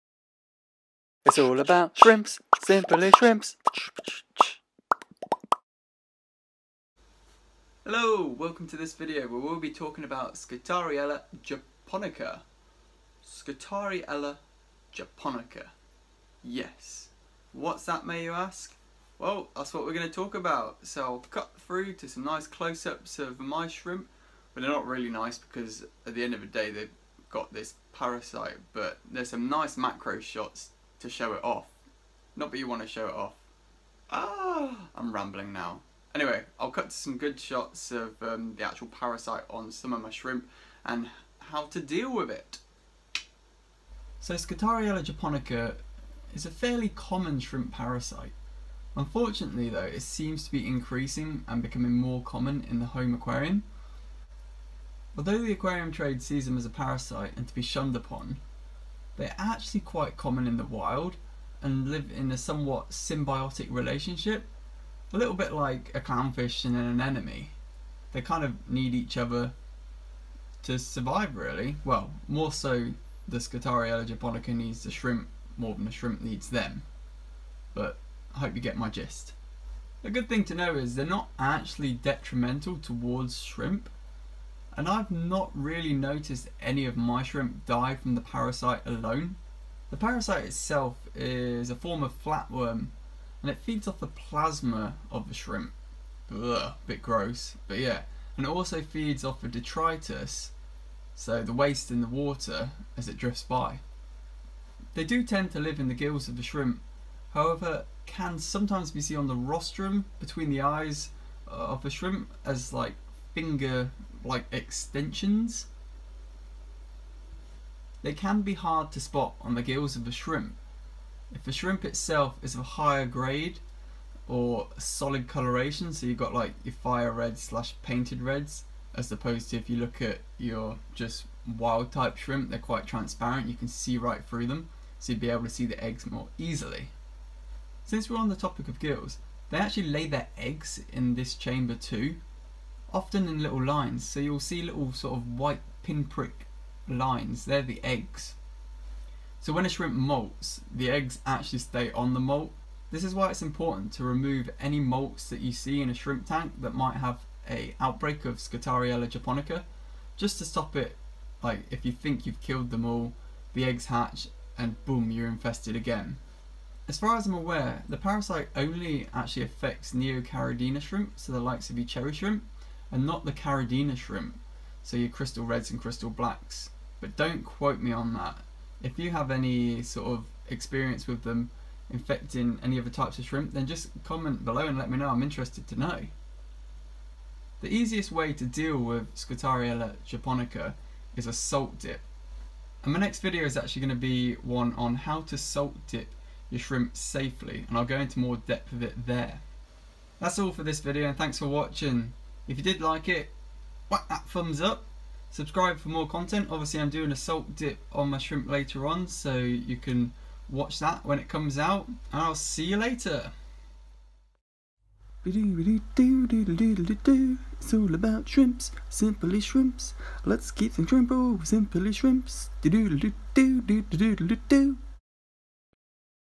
Shrimp Parasite - Scutariella Japonica
Ever wondered what those worm like creatures are on your shrimps? Here's the answer :)

CAMERA GEAR USED:
• My camera
• Camera lens
• SD card
• Vlogging tripod
• Studio lights